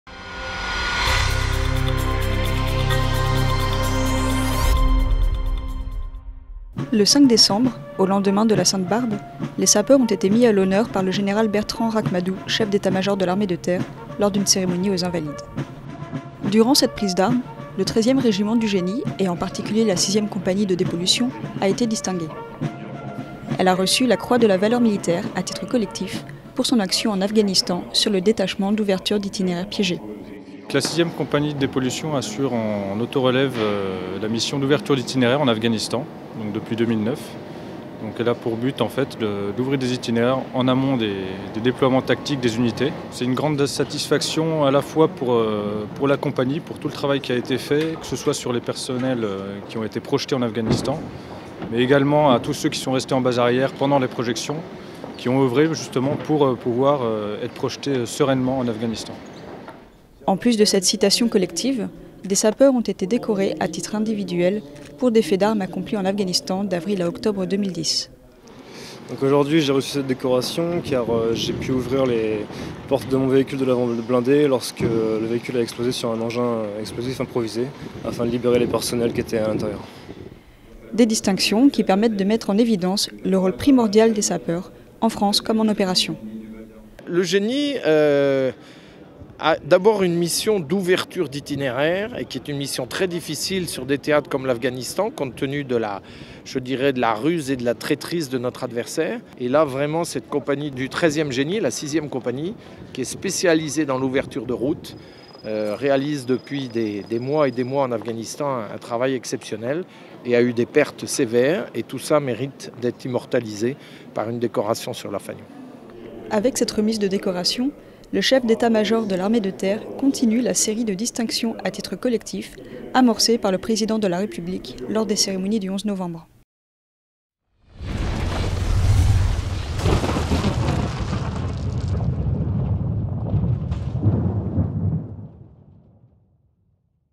Les sapeurs mis à l'honneur
Le lundi 5 décembre 2011, les sapeurs ont été mis à l'honneur par le général Bertrand Ract-Madoux, chef d'Etat-major de l'armée de Terre, lors d'une cérémonie aux Invalides.